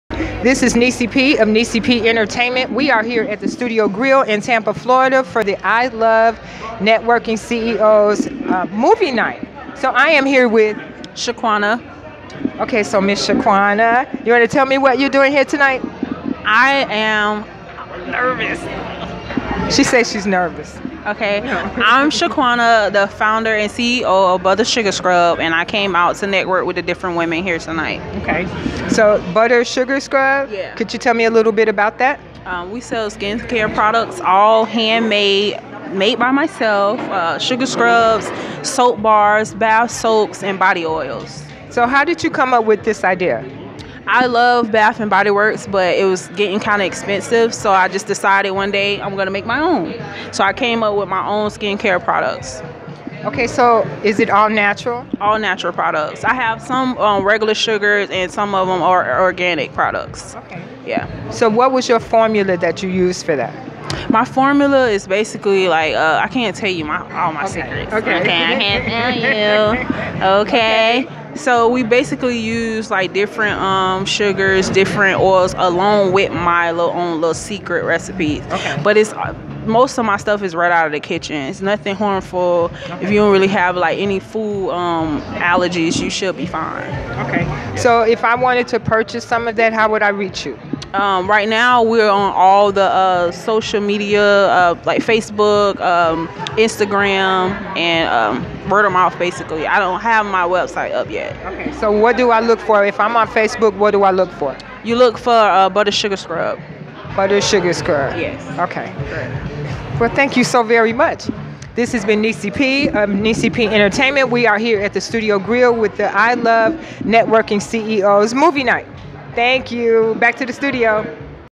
Nessyp with Shaquana Mcfarlin ILWC Networking Movie Night
Nessyp of Nessyp Entertainment with Shaquana Mcfarlin at the I Love Women CEOs networking movie night